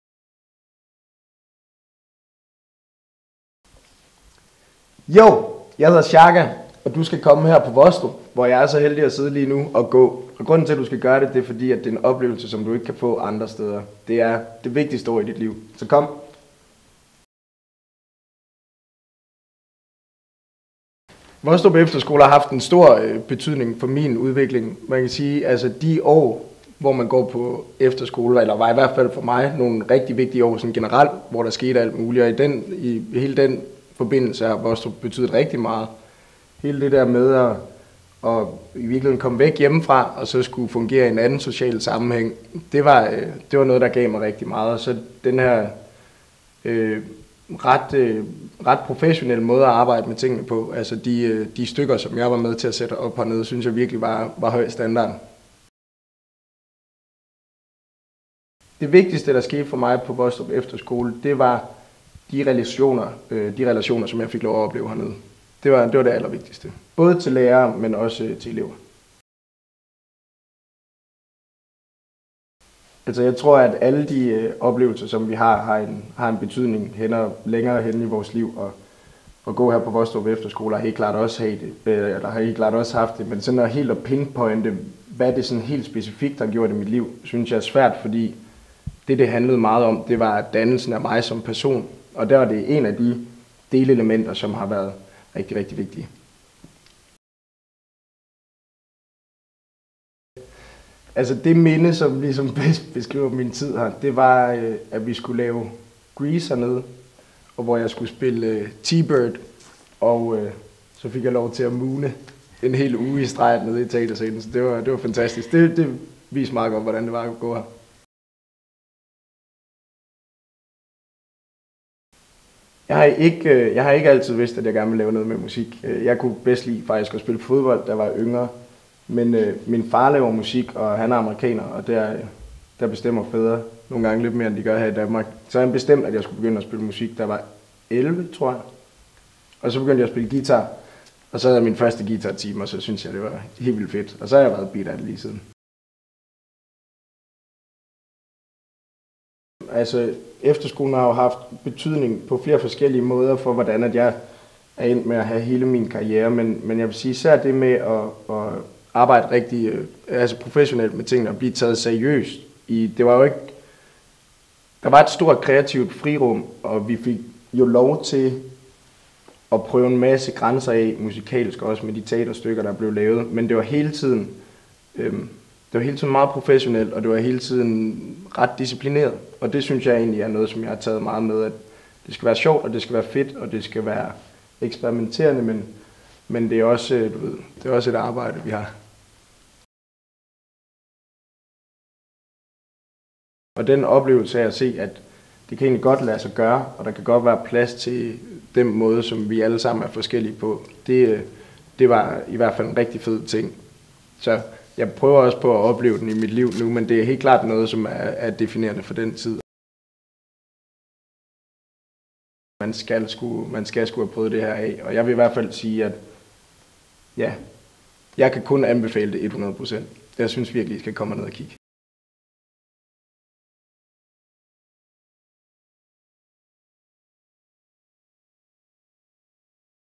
Derfor skal du på Vostrup Efterskole - Shaka Loveless fortæller
Elever på Vostrup Efterskole interviewer tidligere elev Shaka Loveless, som forklarer, hvorfor du skal gå på Vostrup Efterskole.